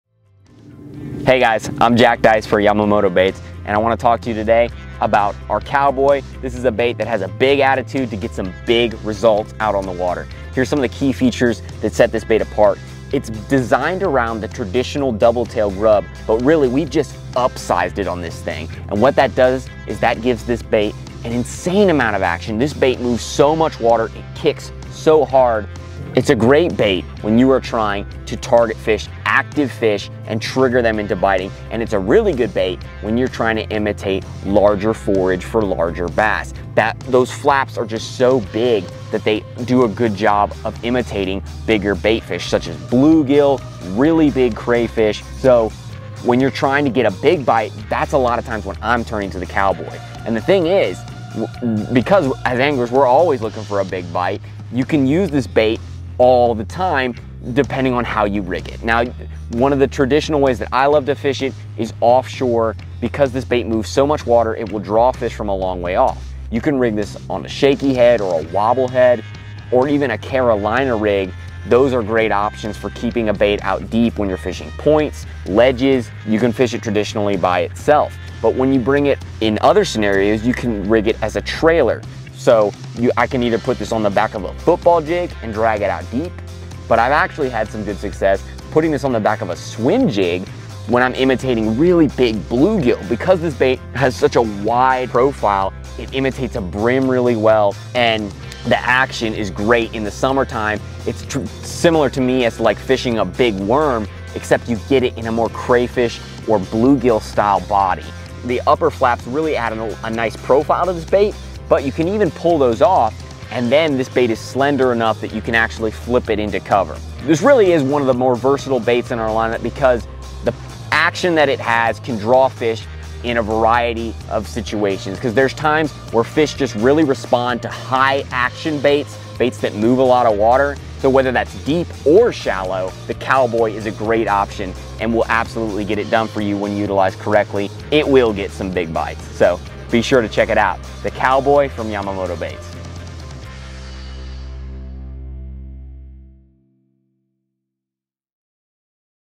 Yamamoto Cowboy
This Cowboy is well-armed. This creature bait pairs the bulky body of the 4.5” Flappin’ Hawg and the swimming tail kick of the iconic DT Grub to create a well-rounded plastic that shines in any scenario. If you need to be shallow, the Cowboy excels as a stand-alone Texas Rig as well as on the back of a jig. Conversely, the Cowboy will do just as much damage out away from the bank as part of a Carolina Rig or on a wobble-head jig. Bulky and loud, the Cowboy is ready for a showdown with big bass!